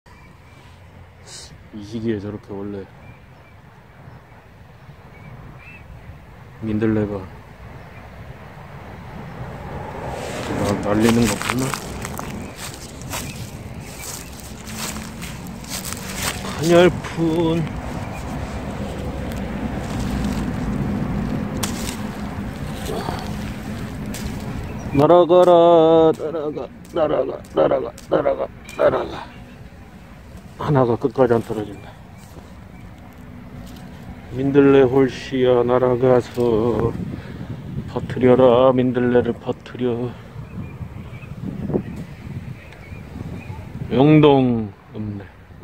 2022년 민들레 씨앗에서 희망을 보다.
인생을 살면서 정말 사소하고 별것 아닌것이 희망이 되고 삶의

동력이 될 수도 있다.

그렇게 믿고 2022년 뭔가 다르게 살아가야겠다.

모든 분들 새해 복많이 받으세요.